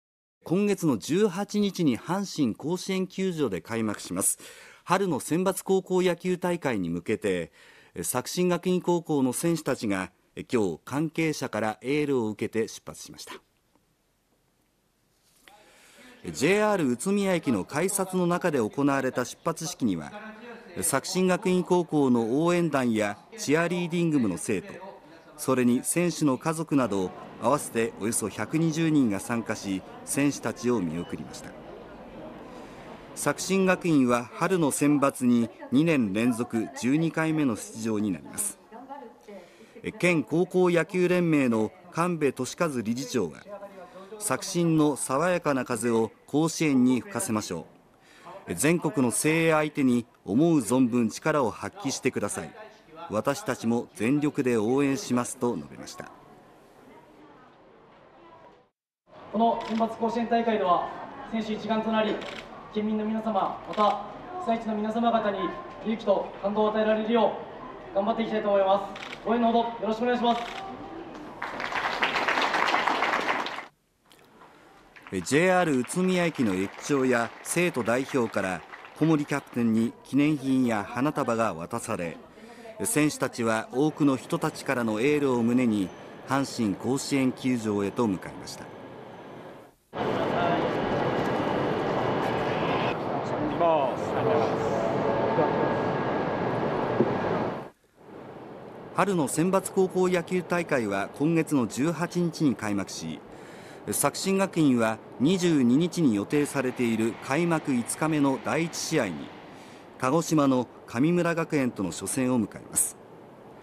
作新学院高校が甲子園へ出発 選手の家族など約１２０人が見送る 春の選抜高校野球大会
３月１８日に阪神甲子園球場で開幕する春の選抜高校野球大会に向けて、作新学院高校の選手たちは１１日に関係者からエールを受けて出発しました。

ＪＲ宇都宮駅の改札の中で行われた出発式には、作新学院高校の応援団やチアリーディング部の生徒、それに選手の家族など合わせておよそ１２０人が参加し、選手たちを見送りました。

作新学院は、春のセンバツに２年連続１２回目の出場になります。

栃木県高校野球連盟の神部知重理事長は「作新のさわやかな風を甲子園に吹かせましょう。全国の精鋭相手に思う存分力を発揮して下さい。私たちも全力で応援します」と述べました。

（作新学院高校　小森一誠主将）
「選手一丸となり、県民の皆様や被災地の皆様方に勇気と感動を与えられるよう頑張っていきたいと思います。応援の程よろしくお願いします」

ＪＲ宇都宮駅の駅長や生徒代表から、小森キャプテンに記念品や花束を渡され、選手たちは多くの人たちからのエールを胸に阪神甲子園球場へと向かいました。

春の選抜高校野球大会は３月１８日に開幕し、作新学院は３月２２日に予定されている開幕５日目の第一試合に、鹿児島の神村学園との初戦を迎えます。